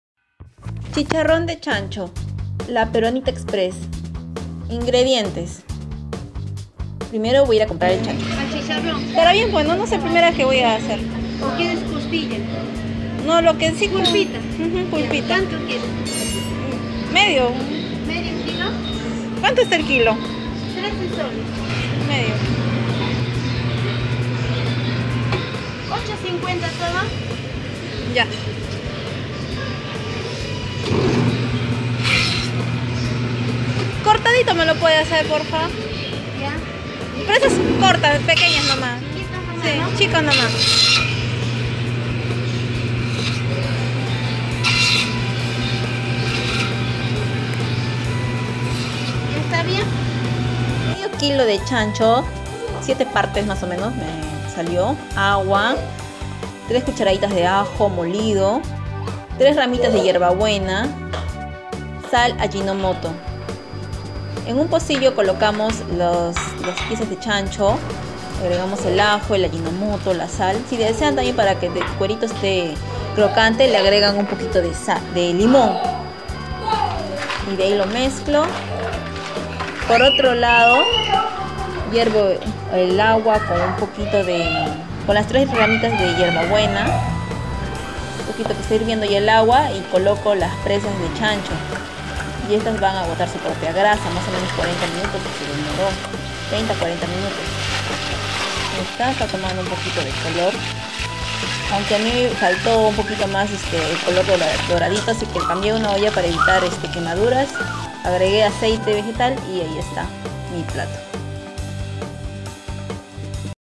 RECETA: Chicharrón de Chancho
INGREDIENTES:
*1/2 KG de chancho torzado
*Ajo, sal, Ajinomoto
*Aceite
*Agua
*4 a 3 ramitas de hierba buena